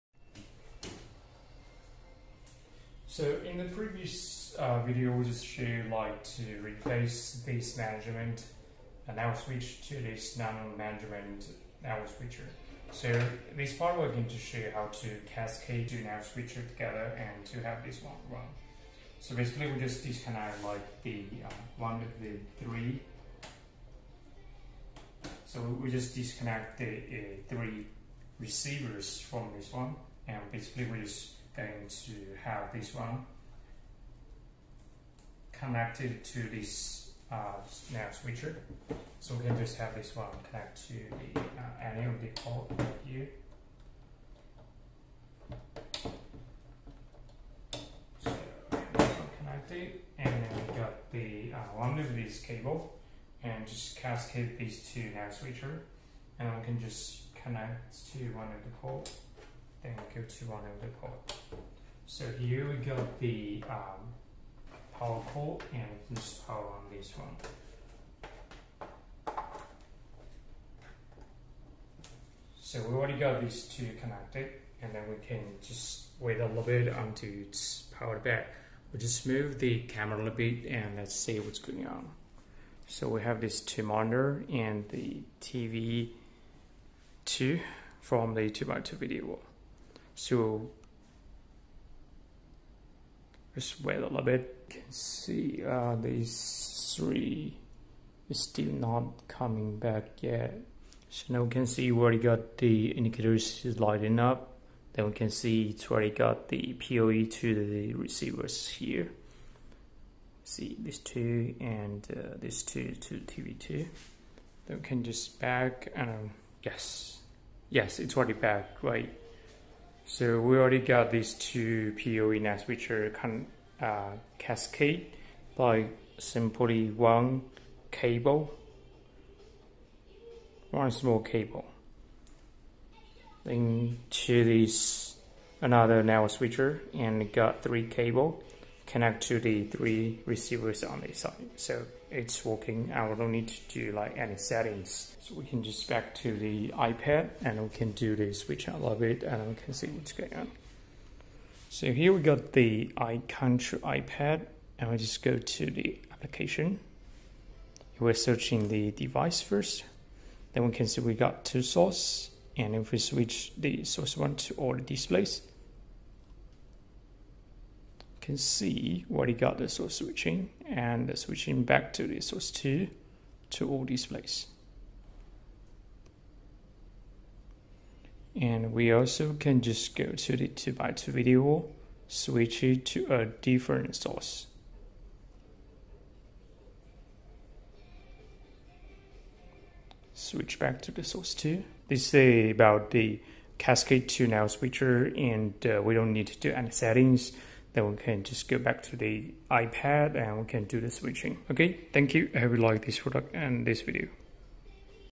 Cascading 2 POE network switch in the AV over IP system
It's a demo video to show how to cascade two POE network switches in the AV over IP system. No need to do any settings, just a cat cable connected the 2 switches.